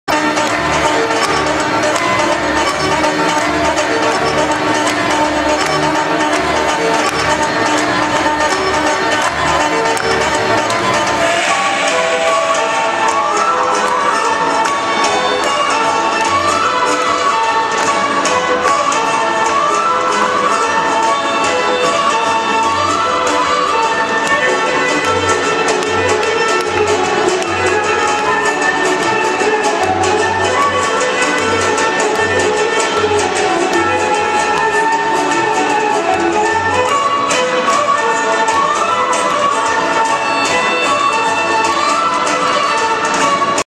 Тф Мини Усмивки - Детски игри
Клипове от участия на възпитаници на Народно читалище "Димитър Динев".